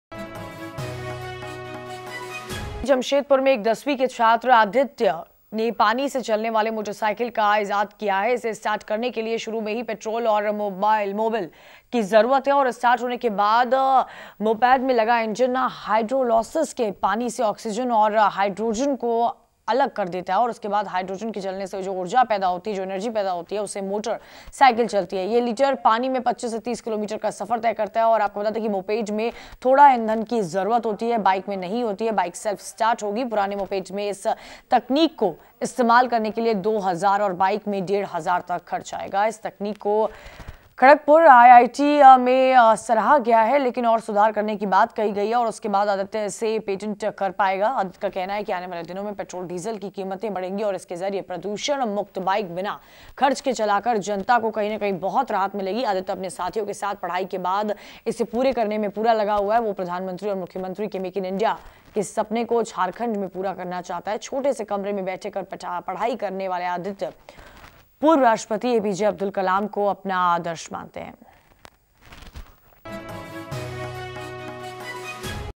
10th class student invents motorbike which can run on water
A class 10th student from Jamshedpur, Aditya Gotte has invented a motorbike which can run up to 25 to 30 Km using six liters of water. Aditya’s invention just needs a small amount of fuel to start the engine.